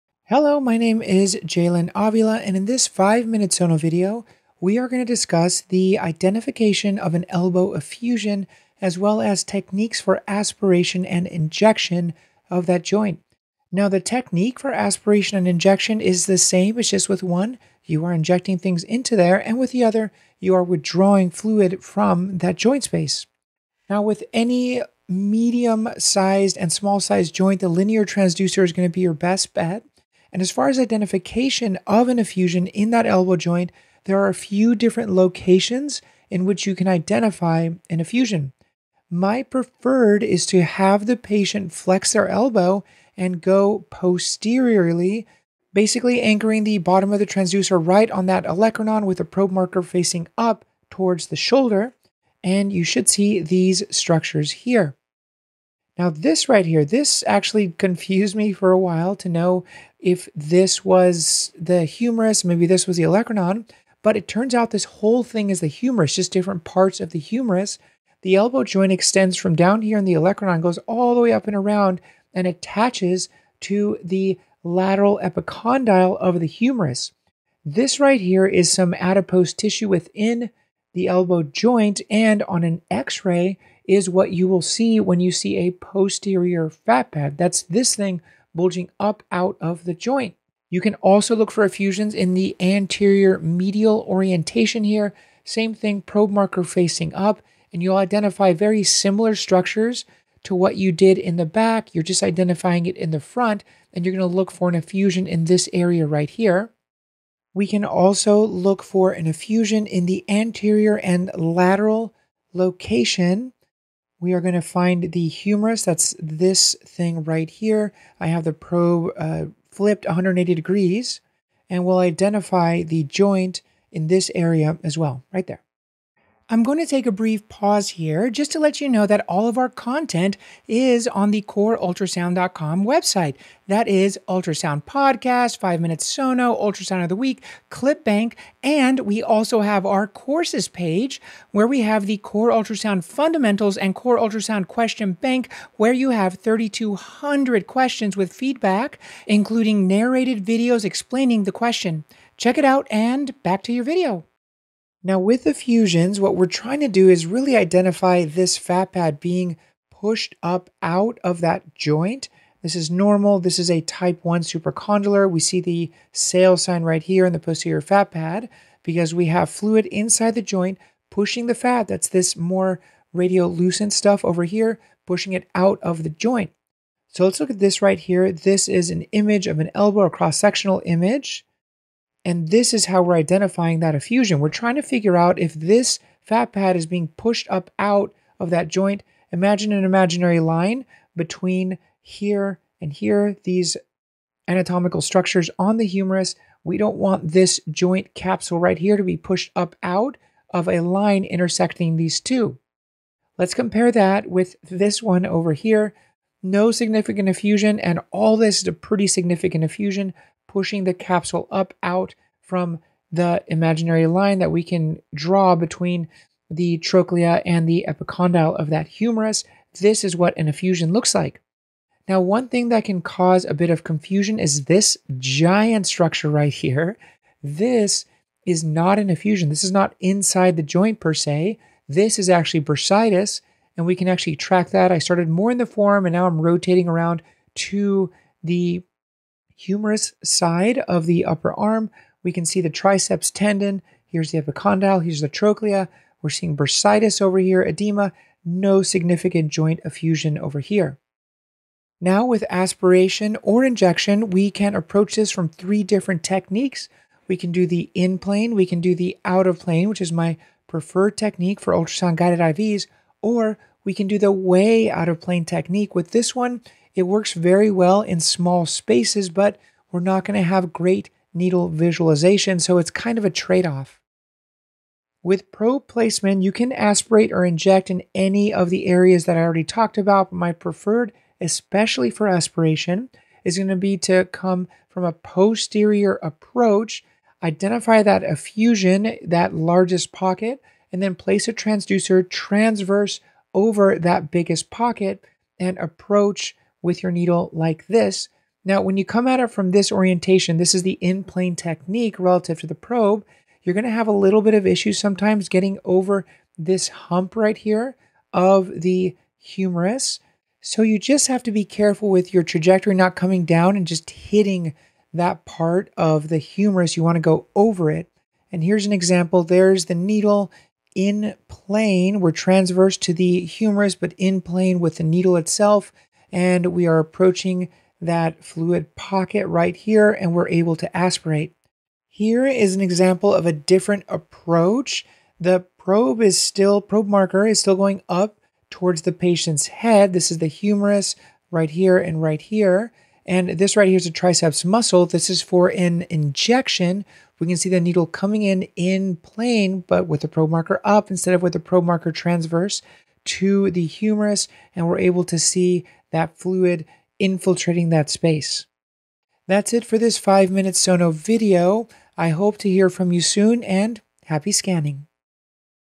Elbow Effusion - Identification, Aspiration and Injection
In this Five Minute Sono video, Jailyn discusses how to identify an elbow effusion, and then how to aspirate fluid or inject medications. This is a potentially very difficult procedure made easier by bedside ultrasound. Check out the video to learn more!

(Of note, this video only discovers the ultrasound identification and needle guidance aspects. Please check with local rules/guidelines/standard of care for exact techniques!)

We have a lot more exciting content coming your way!